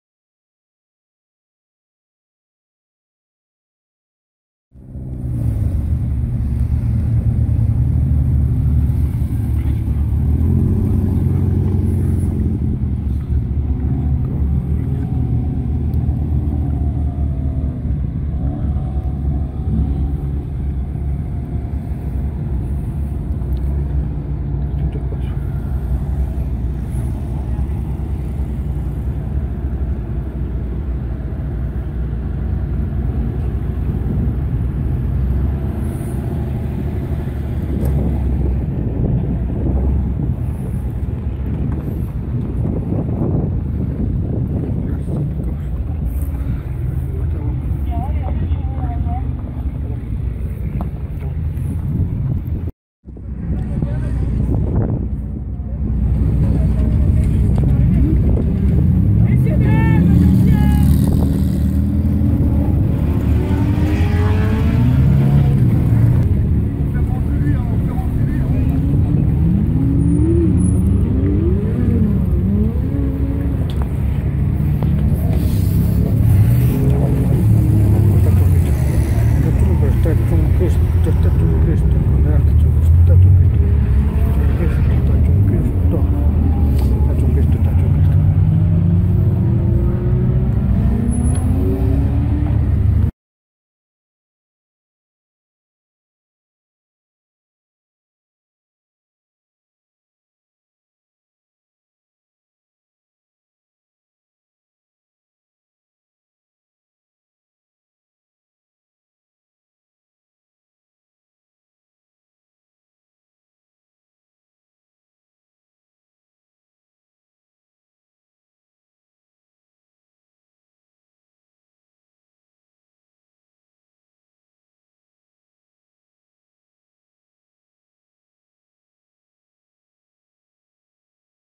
peugeot 304 syc 2024
en este video enseñamos un modelo clasico de peugeot  ,este 304 que estaba en un evento en francia 2024.